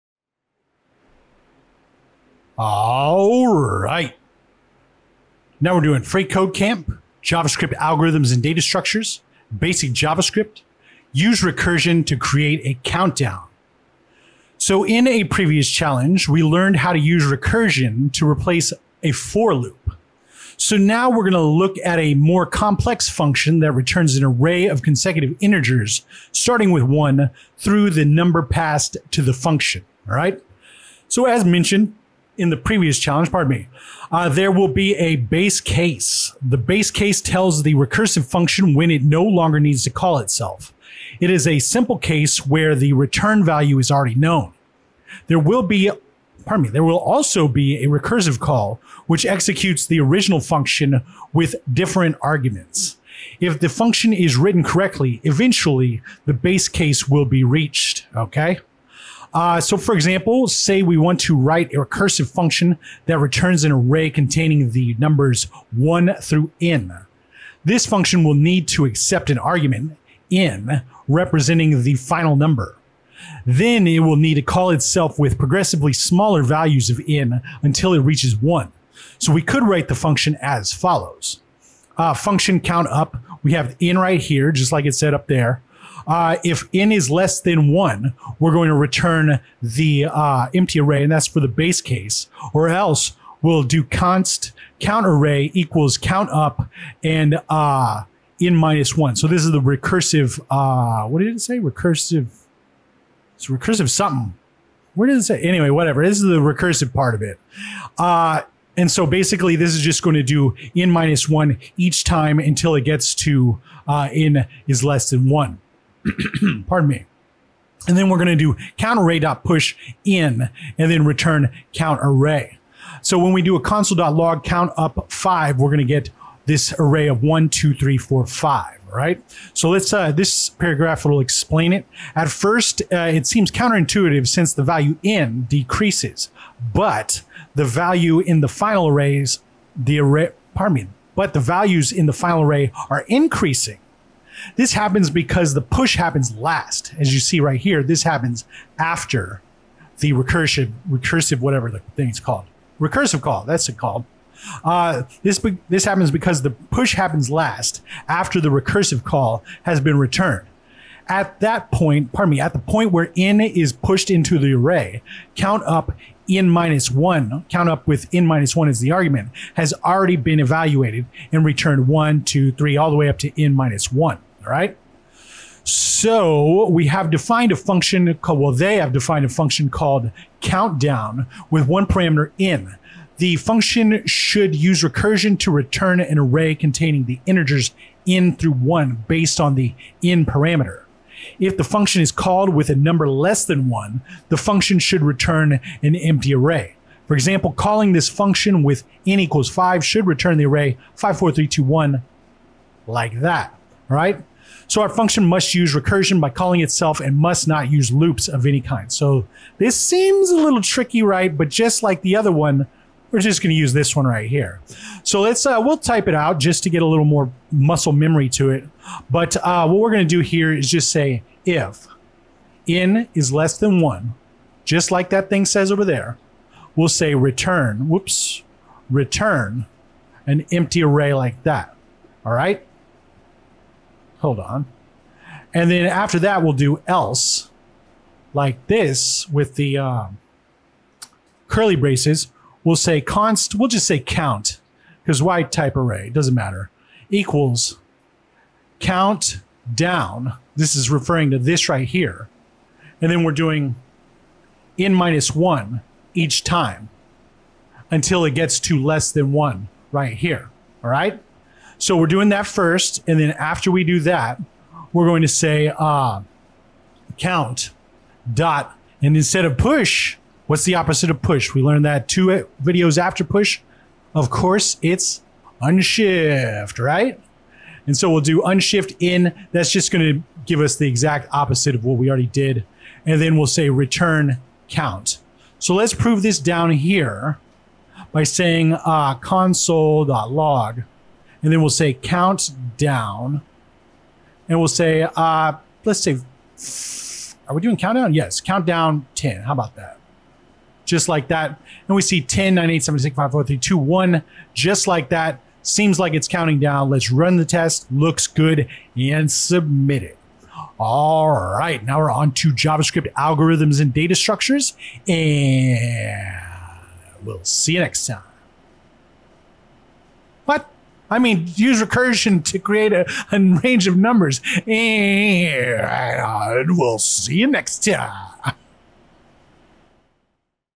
JavaScript Basic 112: Use Recursion to Create a Countdown | FreeCodeCamp
🎉 **"Recursive Revelry: Countdown to Fun-time!"** 🚀🎈

Hey Party Coders and Loop-less Lovers! 🍕🎧

Who needs a New Year's Eve countdown when you've got RECURSION to get the party started?  Prepare to dive into the confetti-filled world of recursion, where we skip the traditional party poppers (loops) and let the recursive magic do the countdown! 🎊✨

🎈 **Highlights from Today's Fun-fiesta**:

- **Back to Basics**: Discover the awesome way of the base case that stops the infinite recursive party! Understand when the music should stop and when the next track (recursive call) should play. 🎵
- **Count UP to Groove Down**: Watch the `countup` function dance its way up from 1 to `n`, sprinkling the array with increasing numbers in a way that seems like a mystery! 🍾🎈
- **Challenge Time**: Join the dance floor and groove with our `countdown` function, making it shimmy from `n` down to 1. Can you make the function return the party numbers in reverse without using those old-fashioned loops? 🎉🕺

By the end, you'll be donning your party hat, holding your array of numbers, and ready to shout: "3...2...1... Recursion Blastoff!" 🚀